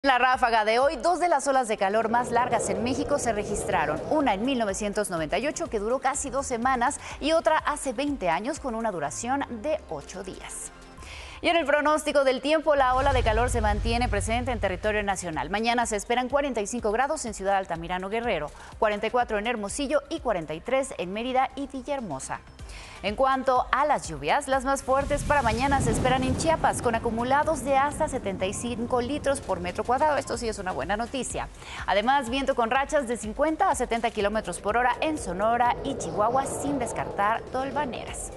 Pronóstico del tiempo 15 de junio de 2023 | Noticias con Crystal Mendivil
¡Tómalo en cuenta! La OlaDeCalor se mantiene presente en territorio nacional. Adriana Trevino te da el pronóstico del tiempo para este jueves.